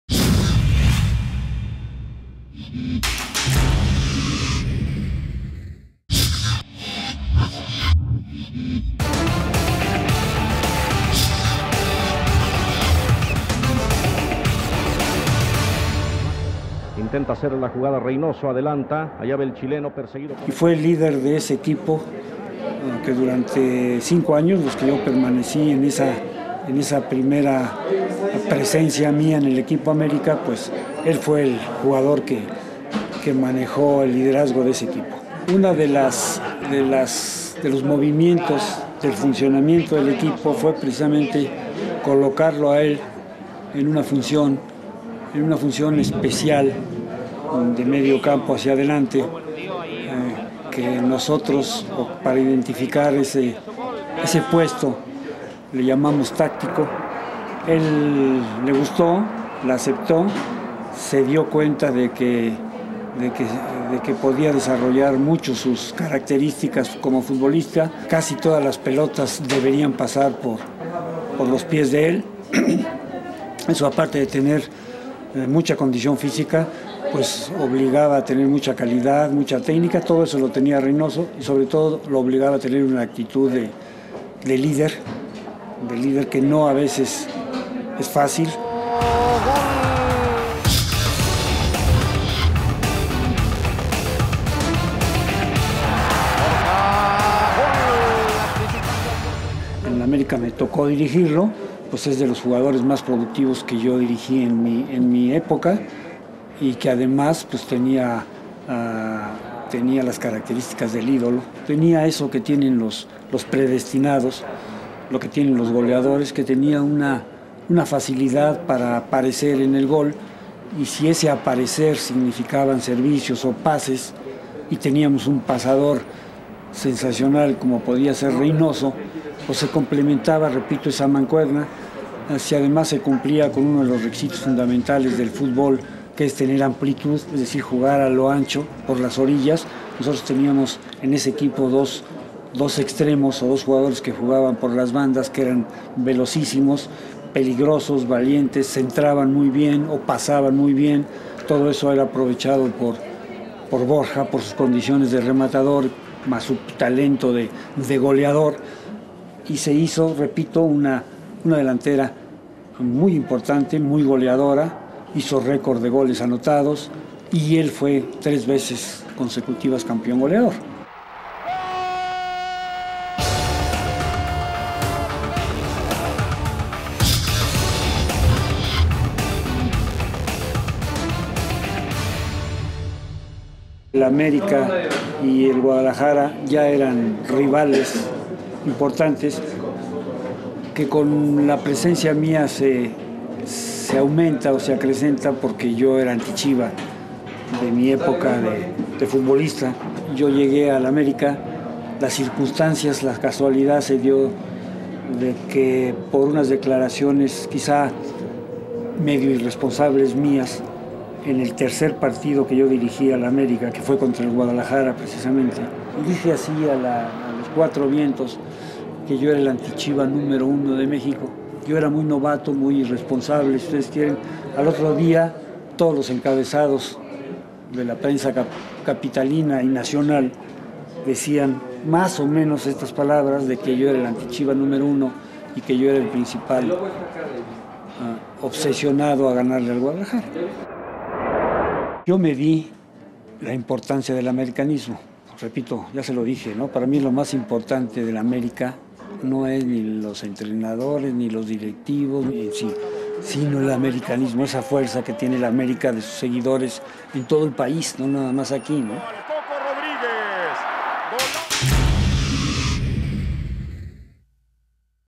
José Antonio Roca. Entrenador histórico del Club América
José Antonio Roca habla del liderazgo de Carlos Reinoso, del instinto goleador de Enrique Borja y de su declaración como el "Antichiva" número uno cuando dirigió a las Águilas del América.